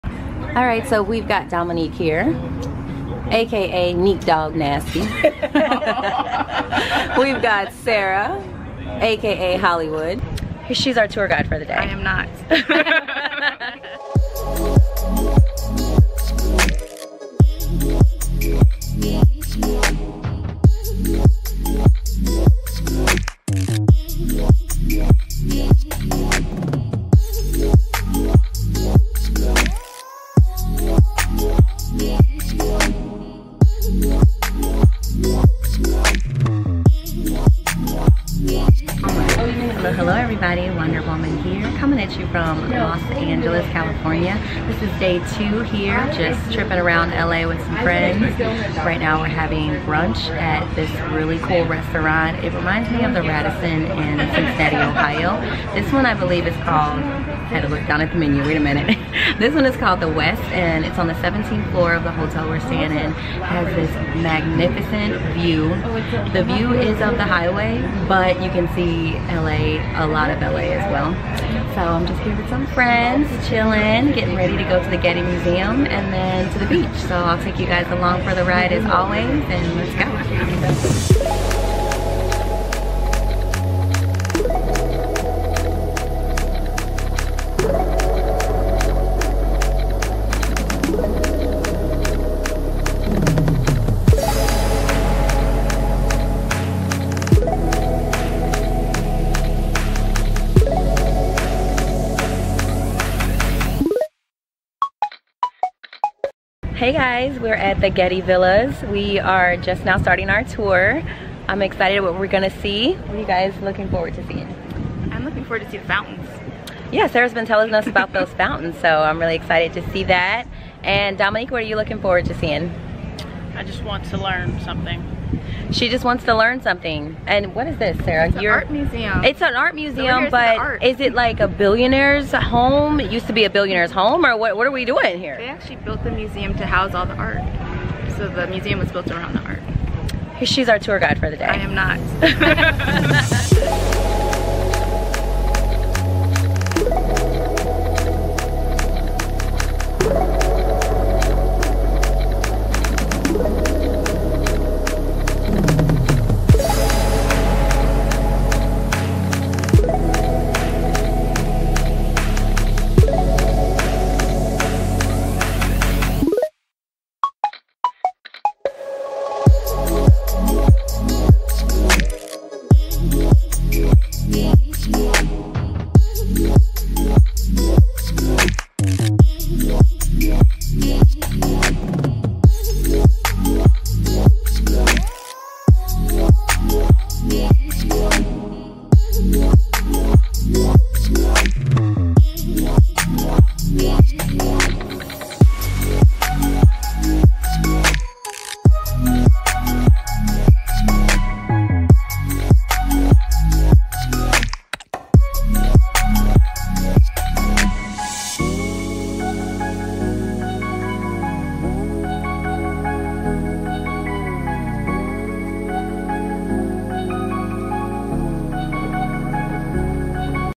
Los Angeles Travel/ Spend the Day With Me
In this video we visited Malibu California and discovered more things to do in LA.

Featuring the Getty Villa, Point Dume Beach and the West restaurant in Hotel Angela on.

spend the Day with me as I roam through this vibrant city.